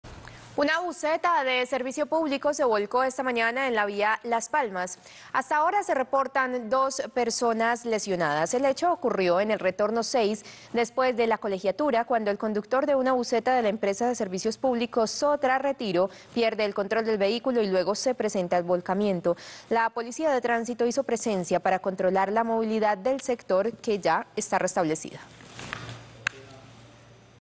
Bus intermunicipal se volcó en vía Las Palmas
Un bus de servicio público de la empresa Sotra Retiro se volcó esta mañana en la vía Las Palmas. La policía de tránsito hizo presencia en la zona. Esto es una noticia en desarrollo.